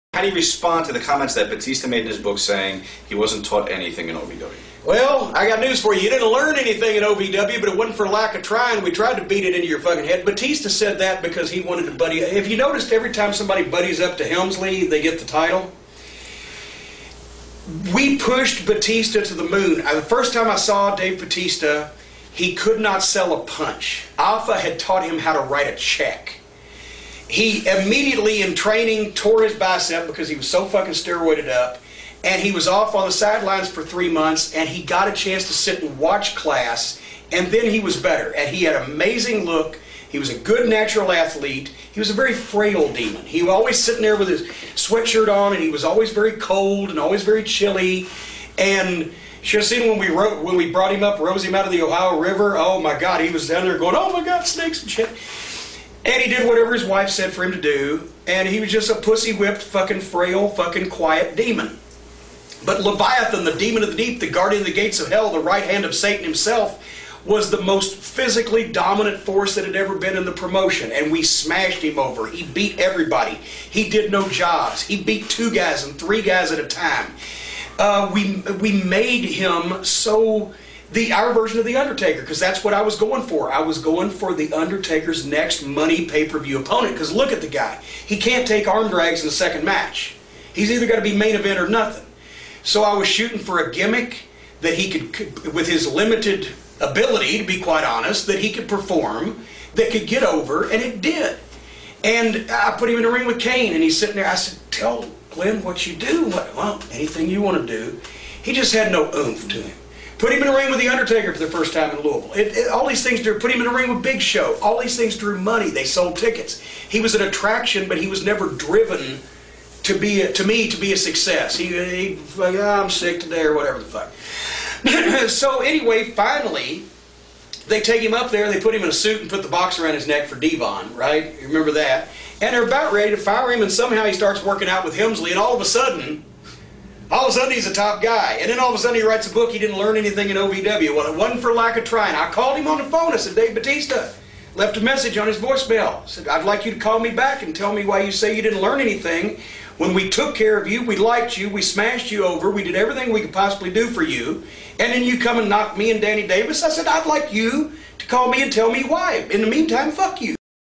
Batista Says He Diddent Learn Anything On OVW
JIM CORNETTE RANTS ON BATISTA SAYING HE DIDDENT LEARN ANYTHING BEING ON OVW BEFORE WWE

#73 - Most Discussed (This Week)) - Nonprofits & Activism - Canada (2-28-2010)

#96 - Most Viewed (This Month)) - Nonprofits & Activism - Canada (March)

BATISTA QUOTES IT ON HIS BOOK